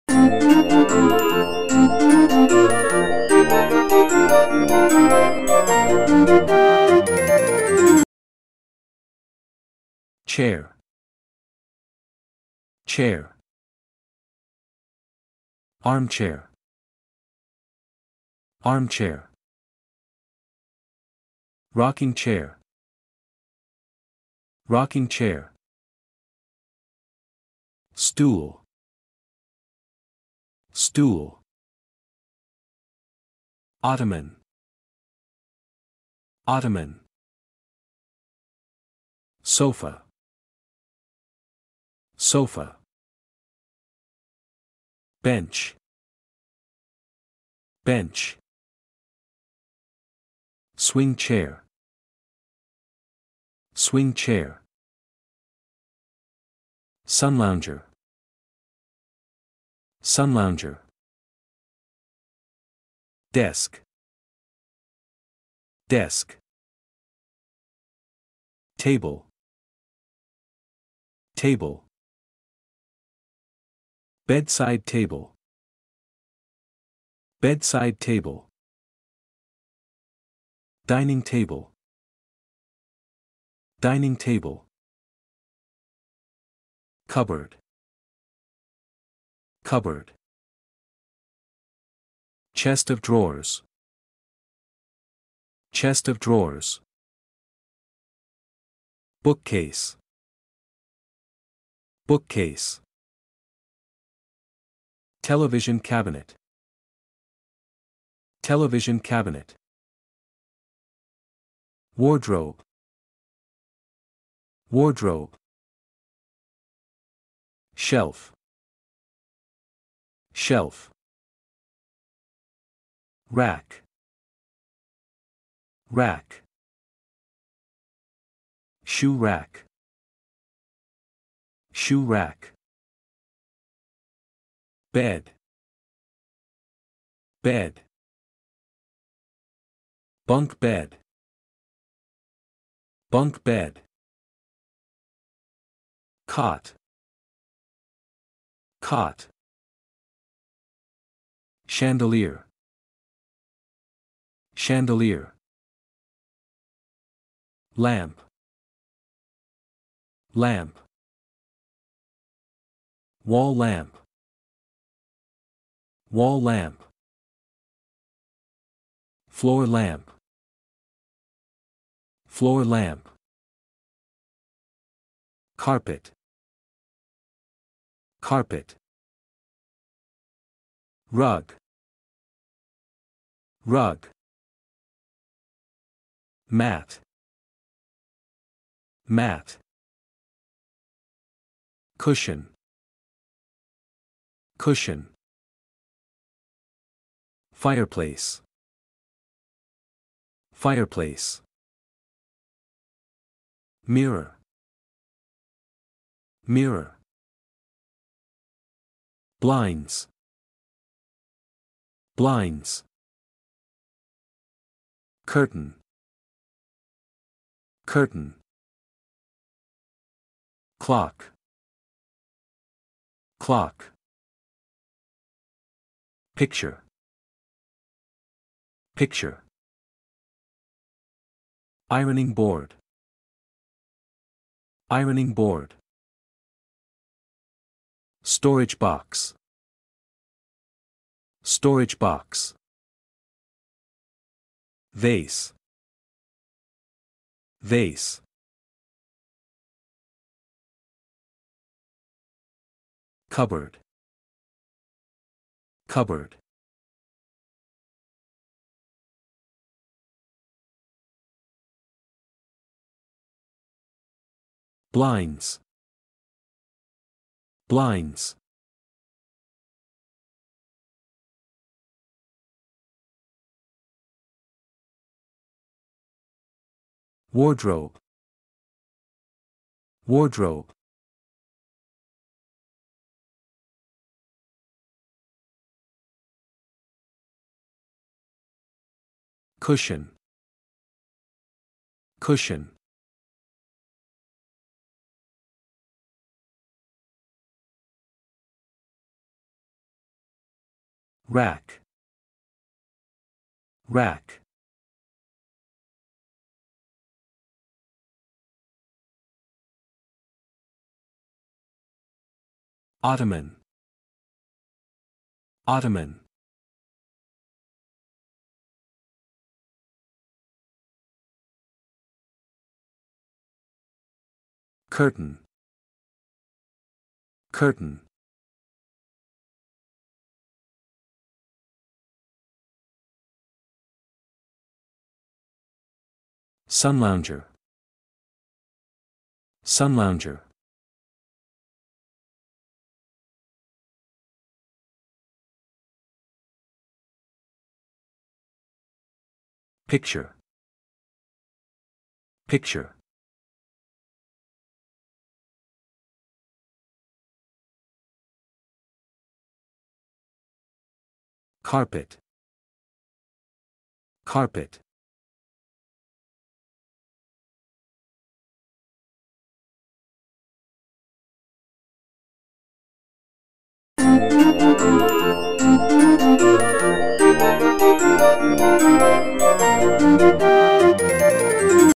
Learning English Vocabulary & Spelling Game: Furniture
Learn furniture in English with pictures.